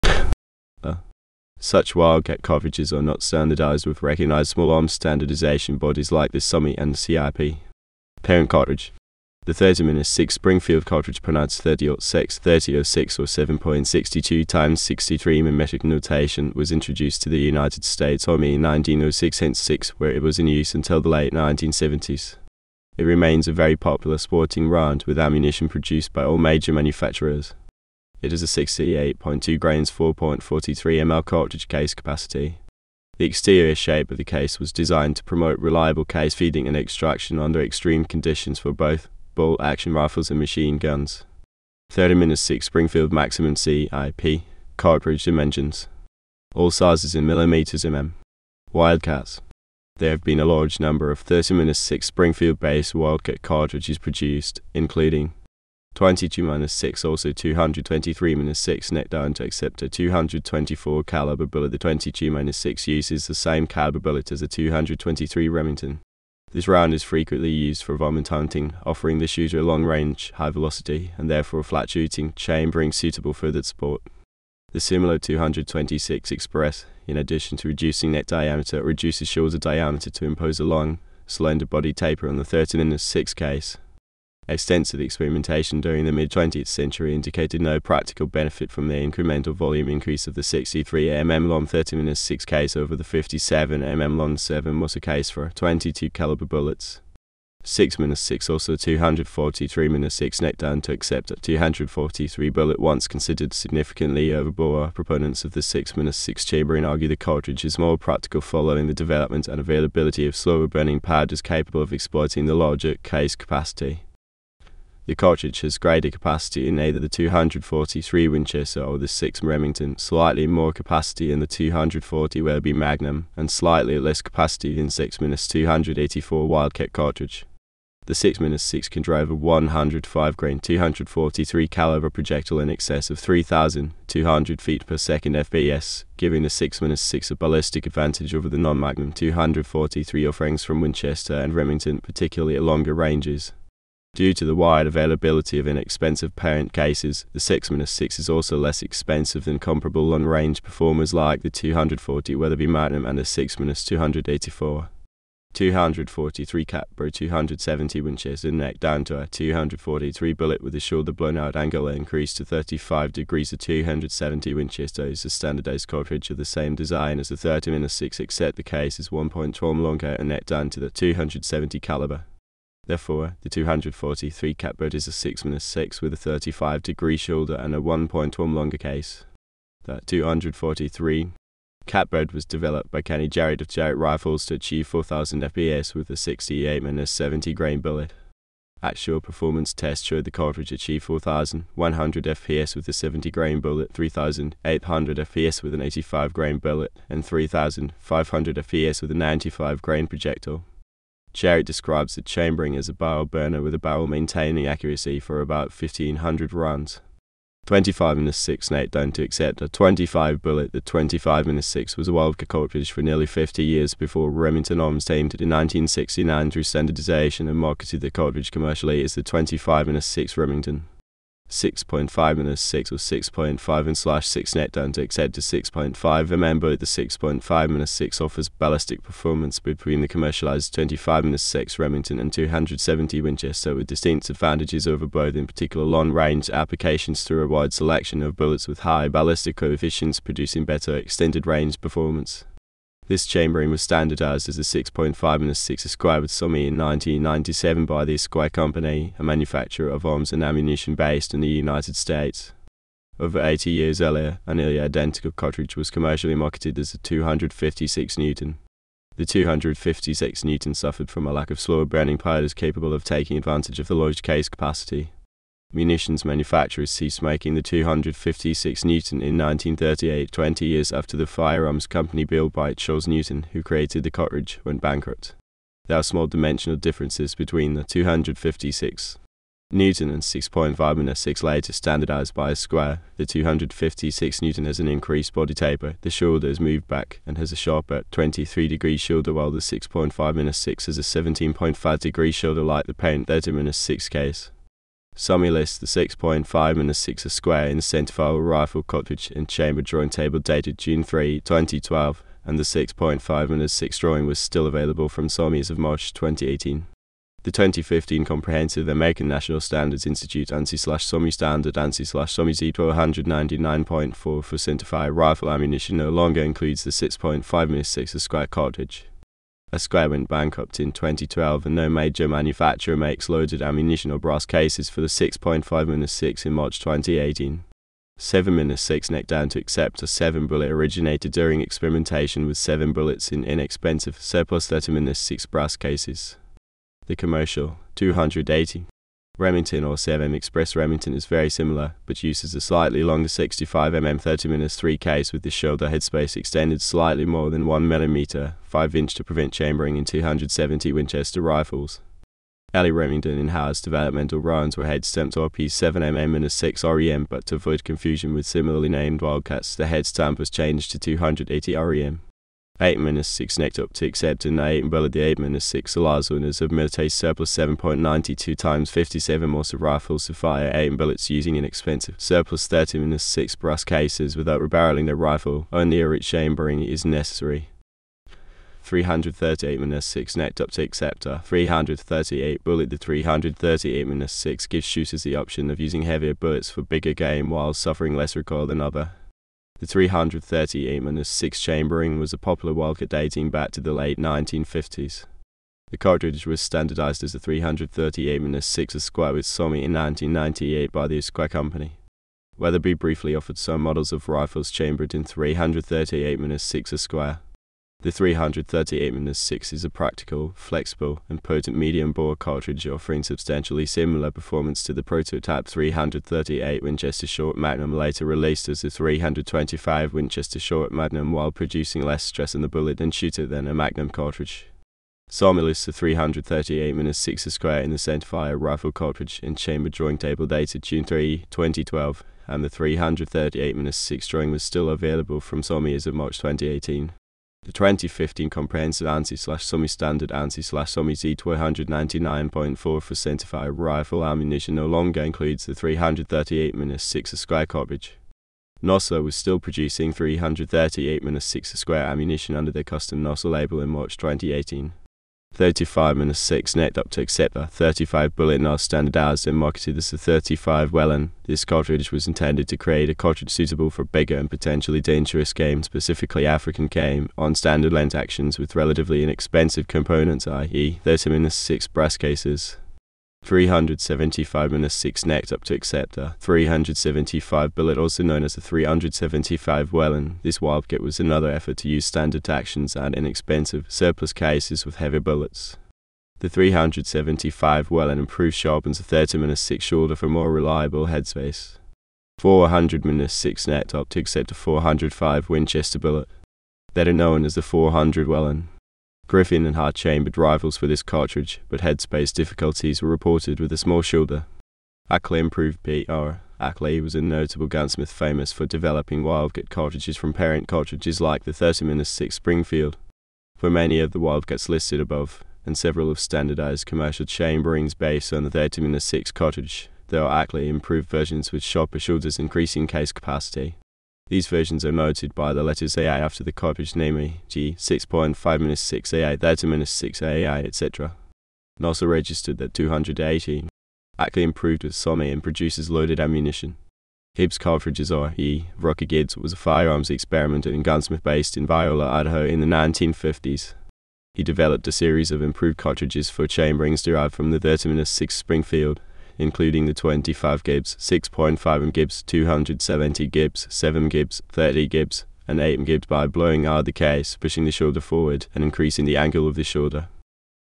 .30-06 Springfield wildcat cartridges
.30-06 Springfield wildcat cartridges are cartridges developed from a 30-06 Springfield "parent cartridge" through narrowing or widening the cartridge neck to fit a smaller or larger bullet in an attempt to improve performance in specific areas. Such wildcat cartridges are not standardized with recognized small arms standardization bodies like the SAAMI and the CIP.